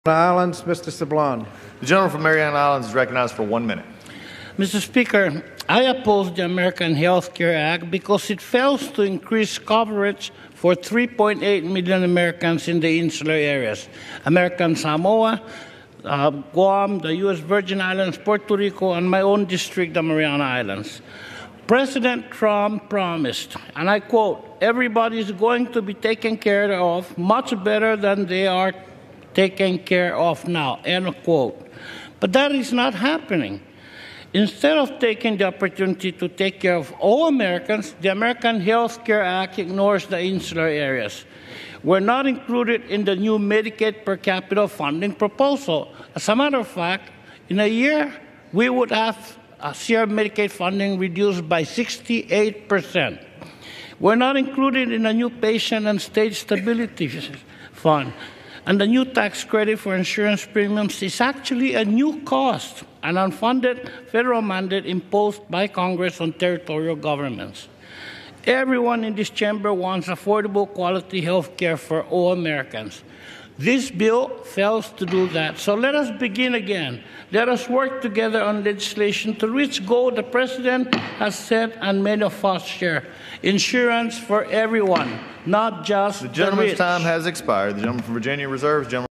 Rep. Sablan Asks Congress Vote No on TrumpCare and "Begin Again"
Rep. Gregorio Sablan (NMI-AL) speaks on the House floor in opposition to TrumpCare because it fails to increase coverage for the Northern Mariana Islands.